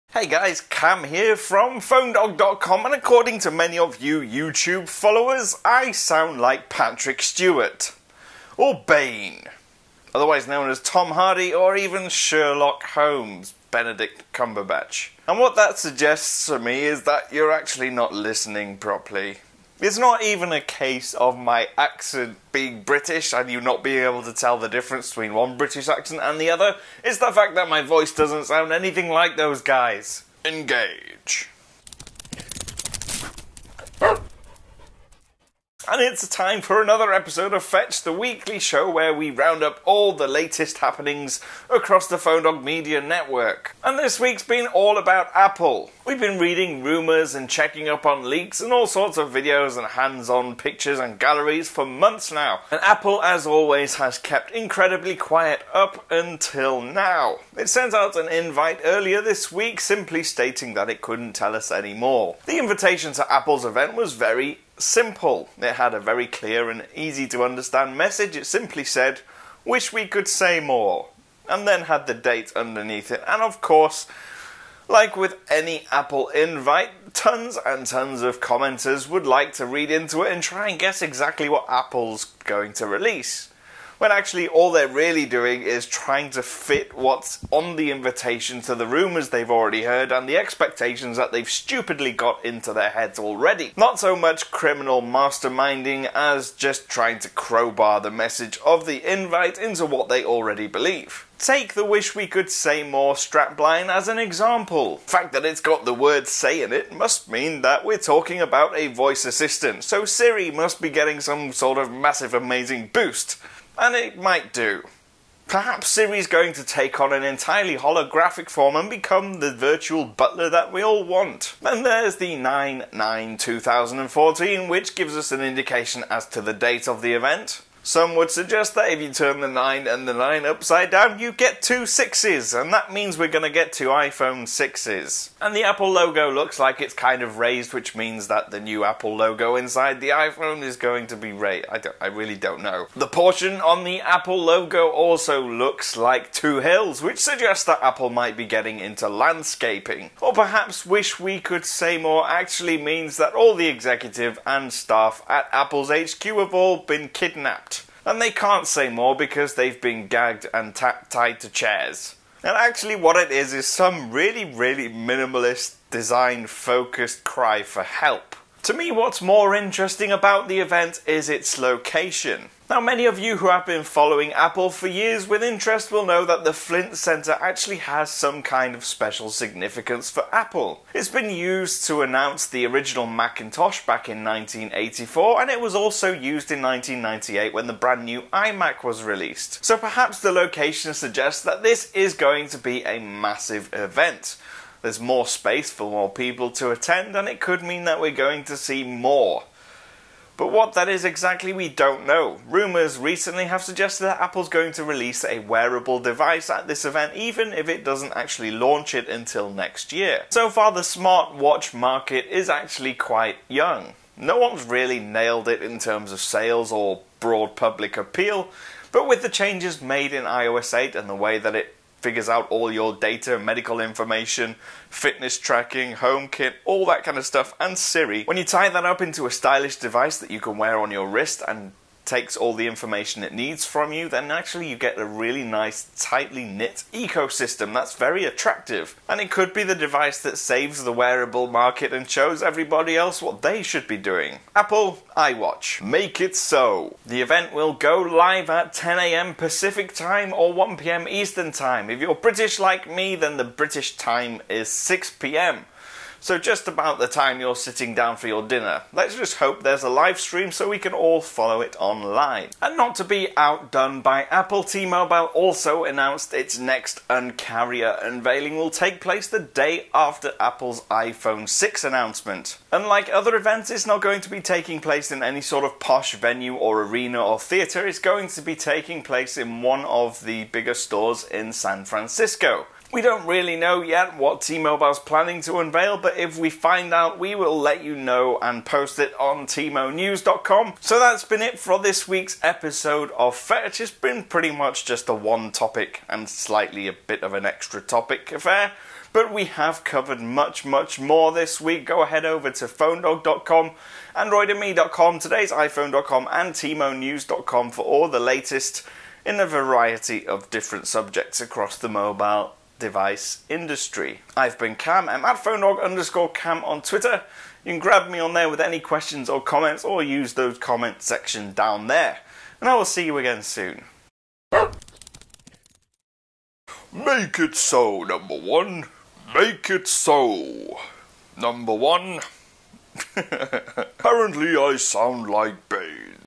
iPhone 6 launch event confirmed, T-Mobile Uncarrier 7.0 incoming!
After months of speculation and rumors, Apple finally confirmed that it will be announcing something on September 9th. Of course, we already know it's going to be the next iPhone. But is there anything in the invite to give us clues, or hints at surprises?

One thing's for sure: The venue has been used for some important products before. Both the original Macintosh and the iMac were announced on the same stage, which hints at how big a deal this next announcement is for Apple. Or does it? We don't really know.

Uncarrier 7.0 for all the latest cell phone news, reviews, and more.